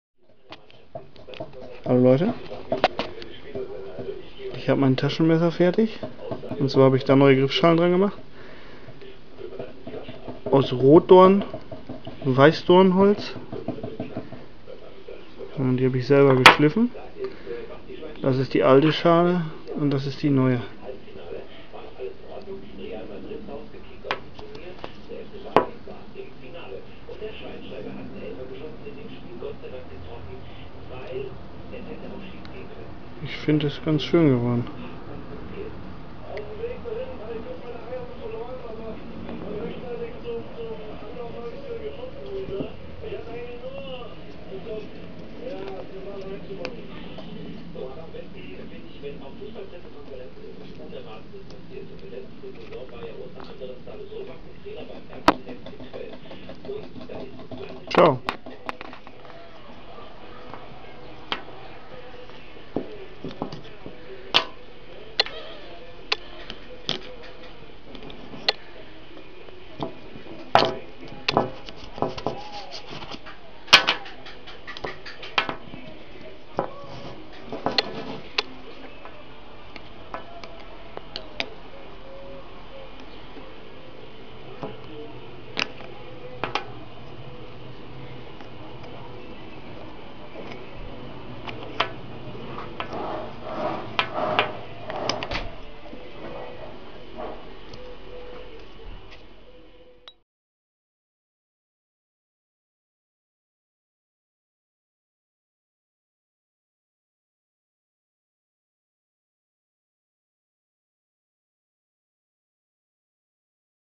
Neue Griffschalen für mein Taschenmesser by INISCH
Hallo Leute,

ich habe bei meinem Taschenmesser die Griffschalen gewechselt. Habe die Griffschalen aus einem getrockneten Rotdornast gemacht. Gesägt, geschliffen und mit Epoxykleber befestigt. Zum Schluss mit Leinöl eingerieben.

LG, Markus